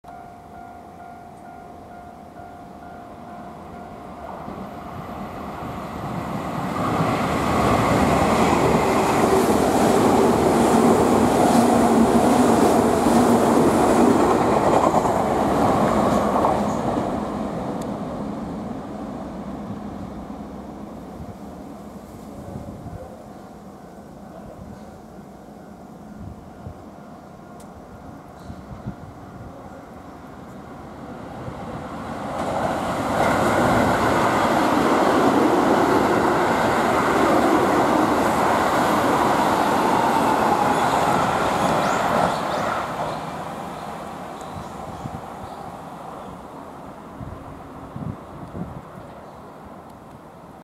〈都営新宿線直通！〉都営10-000形10-280F＆京王9000系9735F 柴崎通過
2017/09/03撮影（撮影日投稿！12時半撮影！）
京王線、柴崎駅を通過する都営10-000形マイナーチェンジ車である8次車の10-280Fです。
快速本八幡行　29T運用でした。
踏切に一眼レフで撮影している方がおりました。
やはり皆さん熱心に撮影してますね（笑）
その後区間急行橋本行の京王9000系9735Fが通過しました。
京王車の都営新宿線直通列車が10-000形と並ぶのもあと僅かです。

※この動画は、1080/60p撮影です。